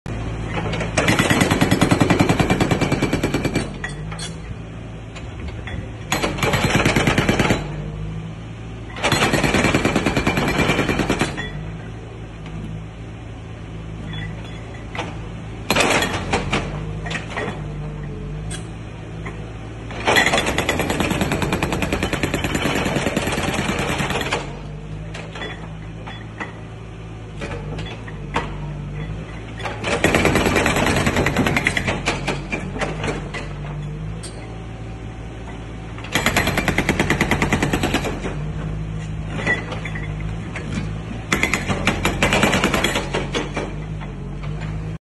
Customer Feedback Video of hmb750 hydraulic Rock Breaker/Stone Breaker concrete pavements
I am very happy to receive the feedback video from customers. The quality of HMB hydraulic breaker has improved the work efficiency of customers and won the reputation.